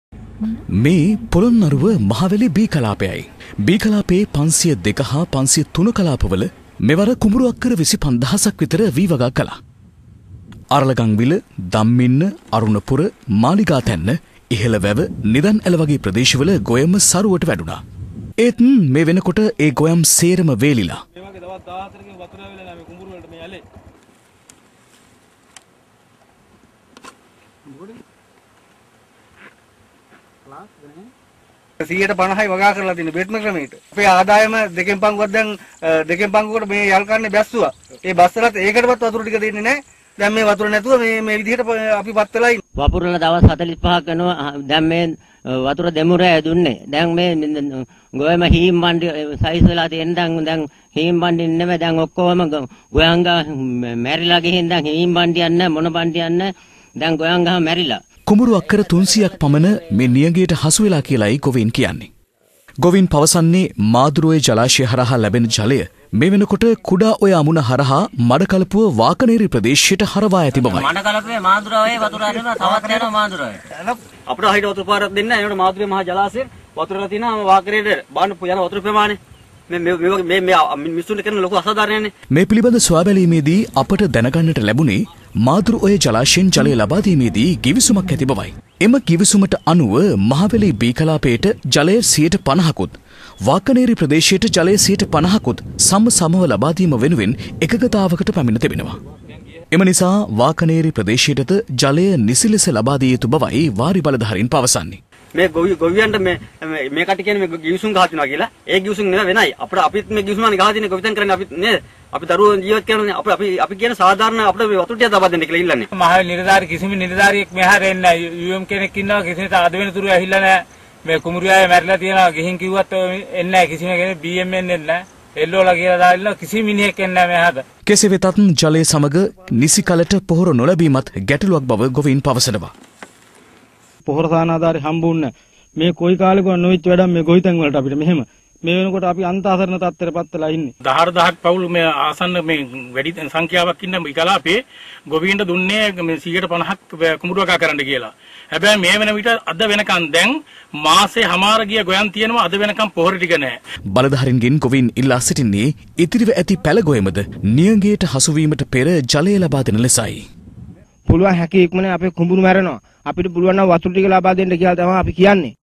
දිවයිනේ ප‍්‍රදේශ කිහිපයකට මහවැසි ඇද හැළුණත් පොළොන්නරුවට වියළි කාළගුණයක්
මේ දිනවල දිවයිනේ ප‍්‍රදේශ කිහිපයකට මහවැසි ඇද හැළුණත් පොළොන්නරුවේ වියළි කාළගුණයක් පවතිනවා. මේ වියළි කාළගුණයත් එක්ක වී වගා කළ ගොවීන් බොහෝ දෙනෙක් අසරණ වෙලා. මේ පොළොන්නරුව මහවැලි බී කලාපයයි. බී කලාපයේ 502 හා 503 කලාප වල මෙවර කුඹුරු අක්කර 25,000 ක් විතර වී වගා කළා.

අරලගංවිල, දම්මින්න, අරුණපුර, මාළිගාතැන්න, ඉහළවැව, නිධන්ඇල වගේ ප‍්‍රදේශවල ගොයම සරුවට වැඩුණා. ඒත් මේ වෙනකොට ඒ ගොයම් සේරම වේලිලා. කුඹුරු අක්කර 300 ක් පමණ මේ නියගයට හසුවෙලා කියලයි ගොවීන් කියන්නේ. ගොවීන් පවසන්නේ මාදුරු ඔය ජලාශය හරහා ලැබෙන ජලය මේ වෙනකොට කුඩා ඔය අමුණ හරහා මඩකලපුව, වාකනේරි ප‍්‍රදේශයට හරවා ඇති බවයි.

මේ පිලිබඳ අප සොයා බැලීමේදී දැනගන්නට ලැබුණේ මාදුරුඔය ජලාශයෙන් ජලය ලබා දීමේදී ගිවිසුමක් ඇති බවයි. එම ගිවිසුමට අනුව මහවැලි බී කලාපයට ජලය සියයට 50 කුත් වාකනේරි ප‍්‍රදේශයට ජලය සියයට 50 කුත් සම සමව ලබා දීම වෙනුවෙන් එකගතාවකට පැමිණ තිබෙනවා.

එම නිසා වාකනේරි ප‍්‍රදේශයටද ජලය නිසි ලෙස ලබා දිය යුතු බවයි වාරි බළධාරීන් පවසන්නේ.කෙසේ වෙතත් ජලය සමගම නිසිකලට පොහොර නොලැබීමත් ගැටලූවක් බව ගොවීන් පවසනවා. බළධාරීන්ගෙන් ගොවීන් ඉල්ලා සිටින්නේ ඉතිරිව ඇති පැළ ගොයම ද නියගයට හසුවීමට පෙර ජලය ලබා දෙන ලෙසයි.